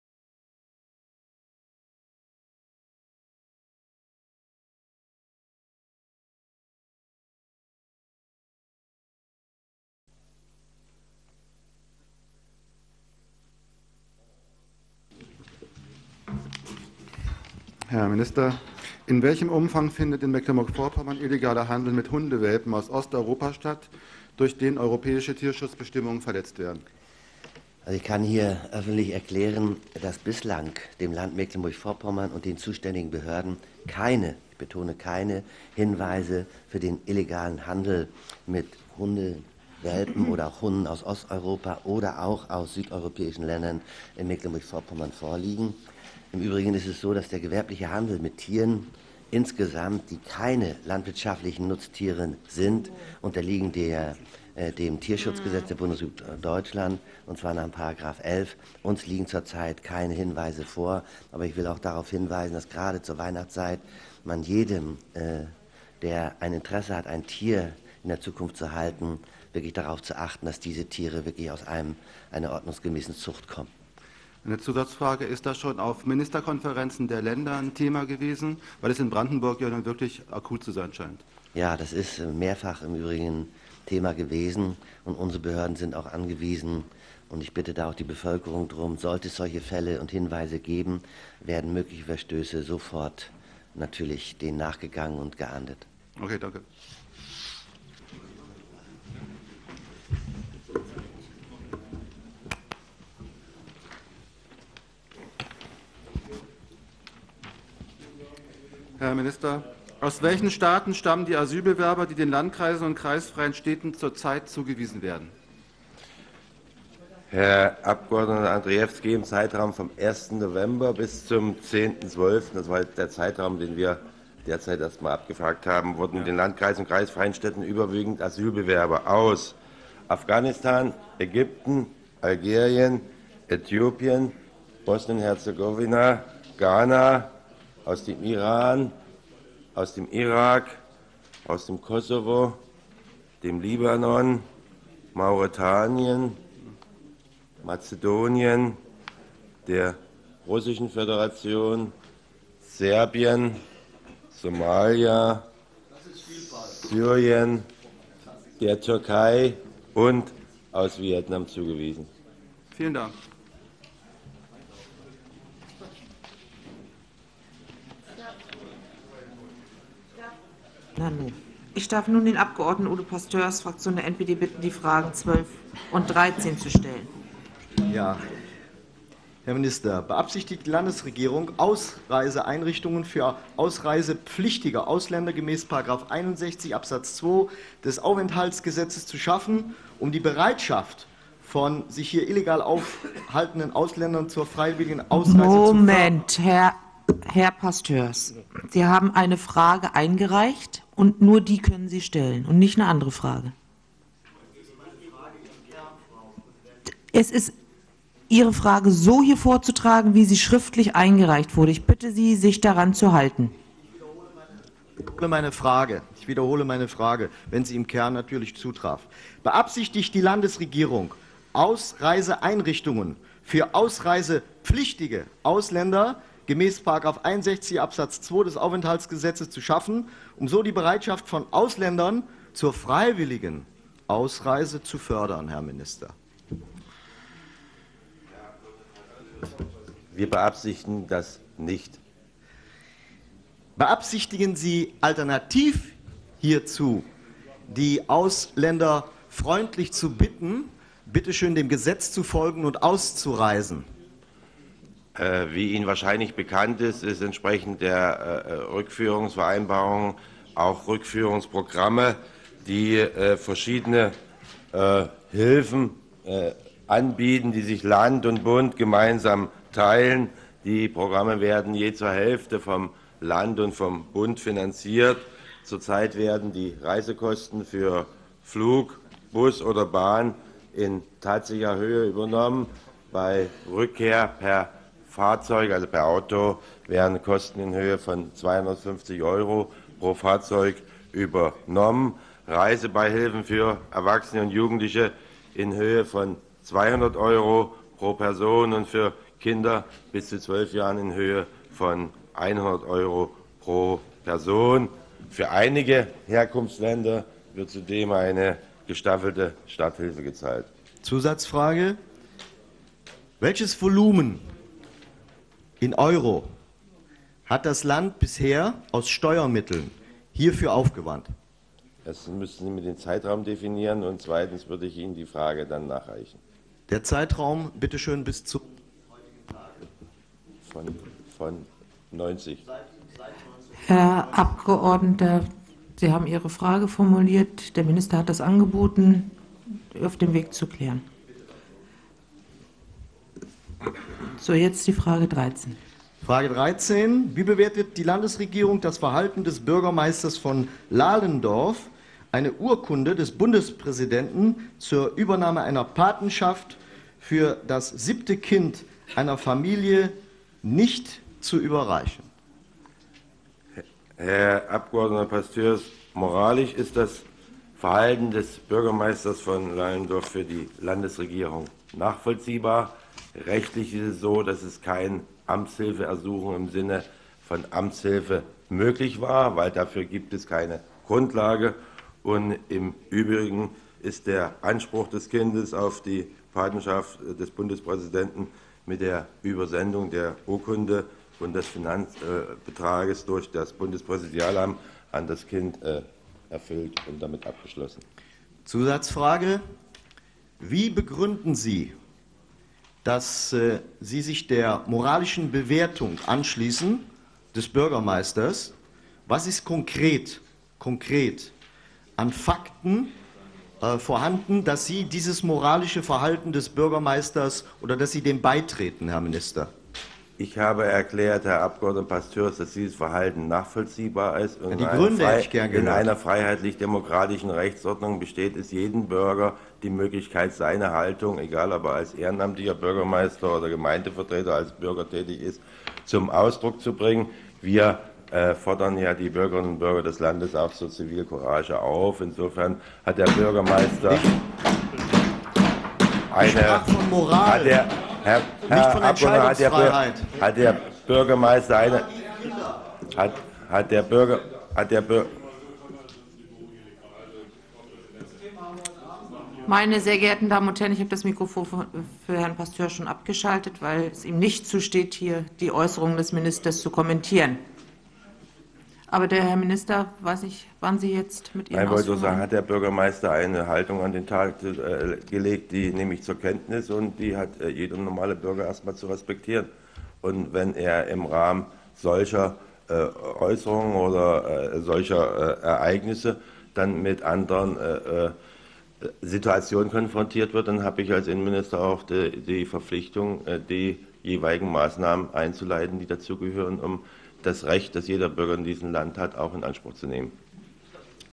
Fragestunde im Landtag: NPD bohrt nach
Auch zu Beginn der heutigen Landtagssitzung ließen die nationalen Volksvertreter im Rahmen der Fragestunde nicht locker. Beispielsweise wollte der NPD-Fraktionsvorsitzende Udo Pastörs von der Landesregierung wissen, wie das mehr als schäbige Verhalten des Lalendorfer Bürgermeisters zu bewerten sei. Bekanntlich hatte sich Herr Knaak (Die Linke) in jüngster Vergangenheit wie ein Politkommissar aufgespielt und sich geweigert, einer volkstreuen Familie die bundesministerielle Ehrenpatenschaft für ihren siebten Sprössling zu überreichen. Auch wollte Udo Pastörs wissen, wie ernst es die Landesregierung mit der Rückführung ausreisepflichtiger Ausländer in der Praxis handhabt.

Der nationale Abgeordnete Michael Andrejewski stellte zudem Fragen zum Tierschutz und Fragen zur Absicht der Landespolitik, Mecklenburg und Pommern gezielt mit Asylanten zu überfluten.